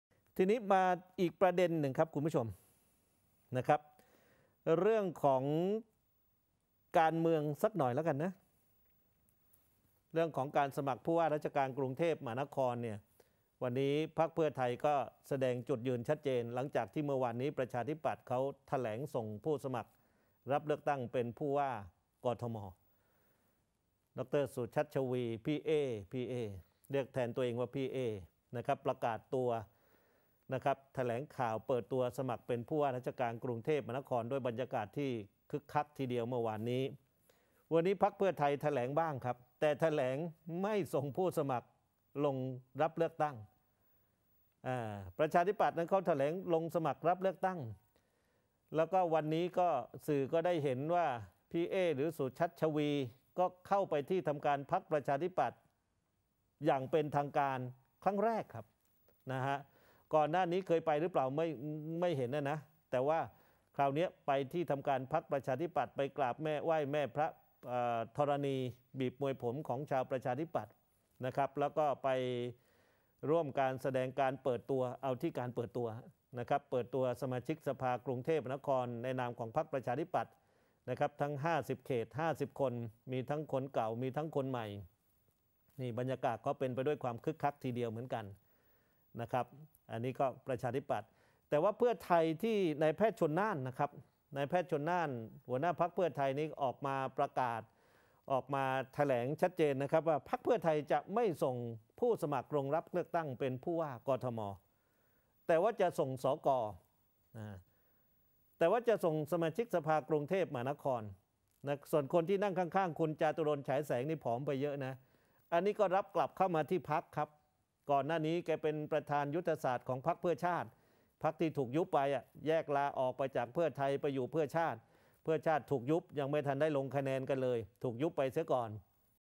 พี่เอ้ สุชัชวีร์ เข้าพรรค ปชป เปิด 50 ว่าที่ สก คึกคัก
ฟังชัดๆ ถนอมจัดให้ 14 ธ.ค. 64